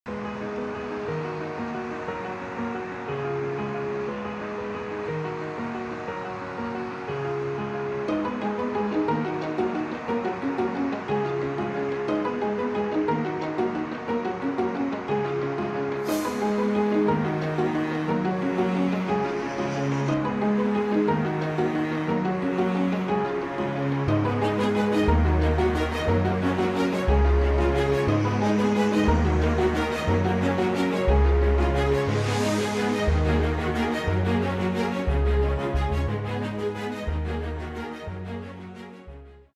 Yola düşen bayrağı gören genç canını tehlikeye attı
İstanbul'da caddeye düşen Türk bayrağını gören genç kendini tehlikeye attı, koşarak bayrağı yerden kaldırdı. O anlar cep telefonu kamerasına yansıdı.